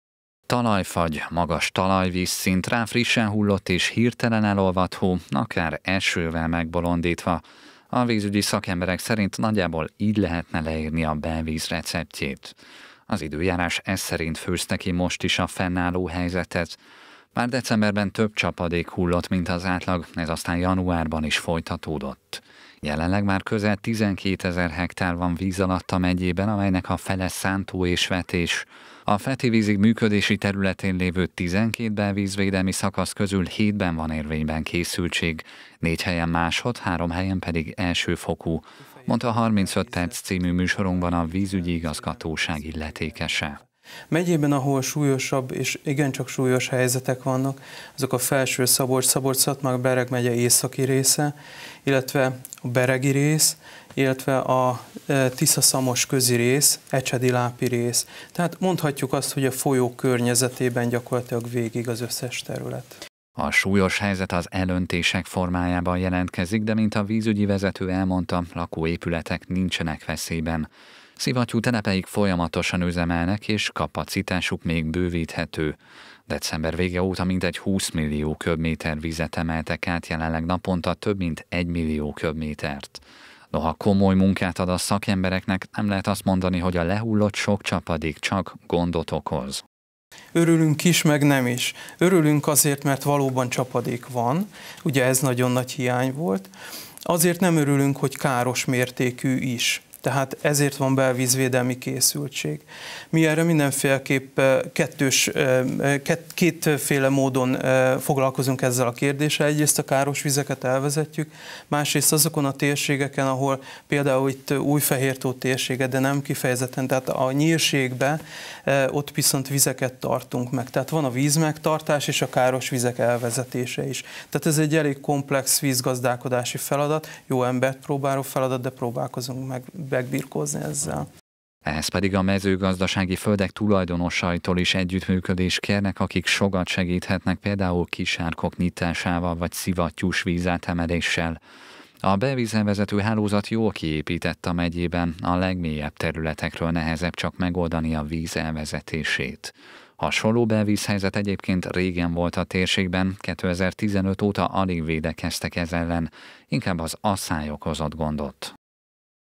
Belvízveszély
Több mint tízezer hektárt borít belvíz Szabolcs-Szatmár-Bereg megyében, de területenként változó, hol mennyire okoz gondot a leesett csapadék. Több helyen is belvízvédelmi készültséget kellett elrendelni, de lakóterület nincsen veszélyben. A Felső-Tisza Vidéki Vízügyi Igazgatóság a mezőgazdaságban dolgozók segítségét is kéri a helyzet kezelésében.